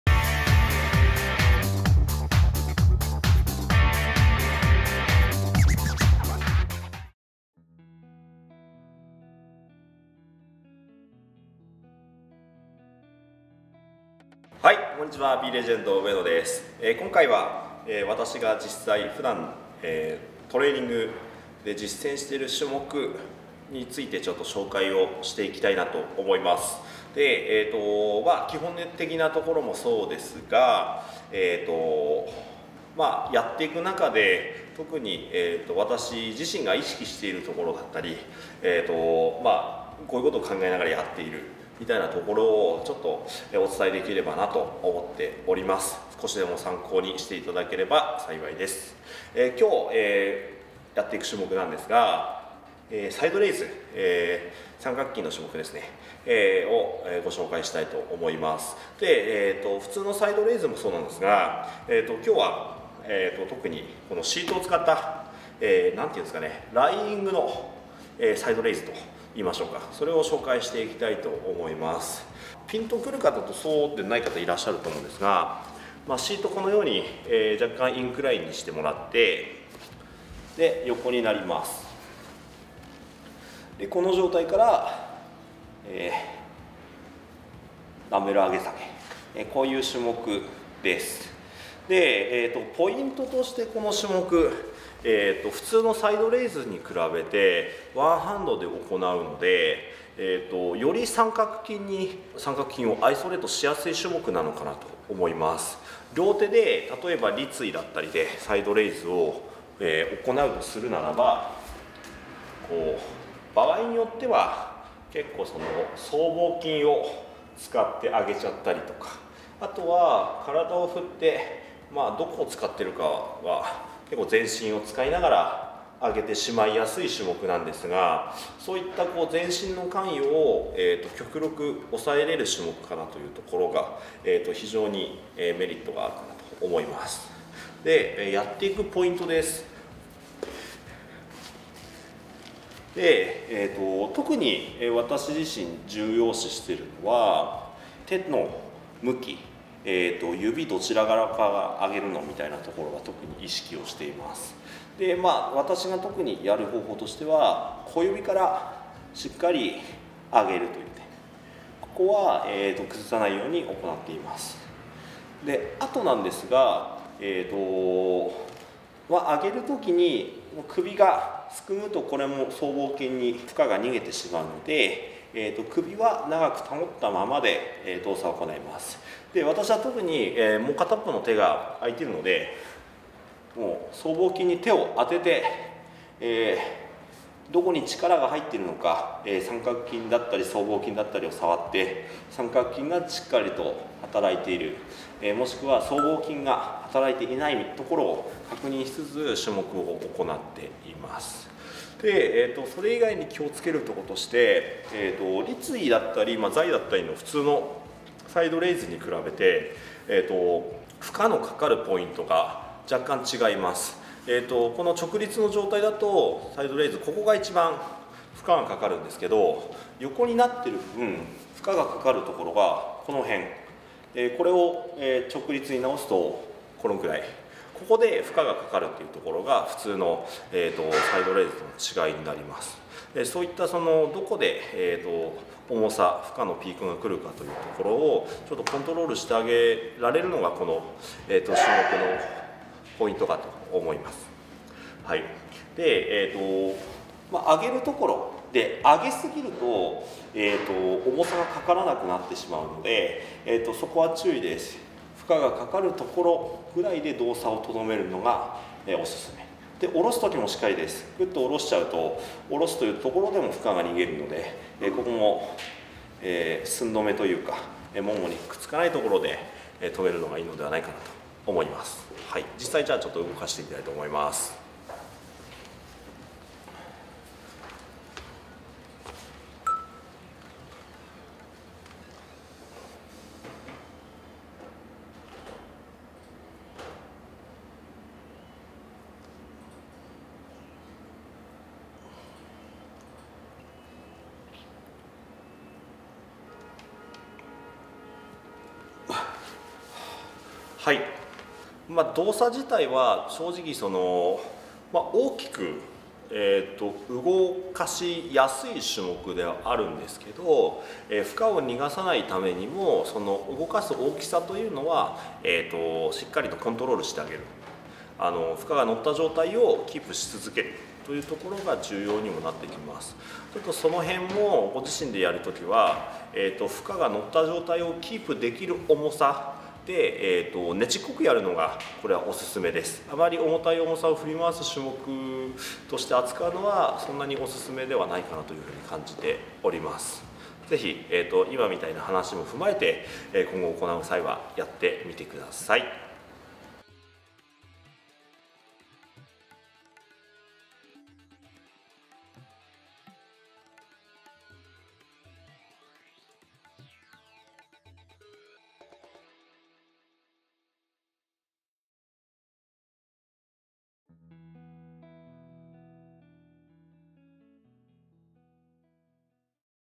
【肩トレ】デカくて丸い肩を作る！ライイングサイドレイズを解説【ビーレジェンド プロテイン】
■ビーレジェンド プロテインバーの詳細はコチラ⇒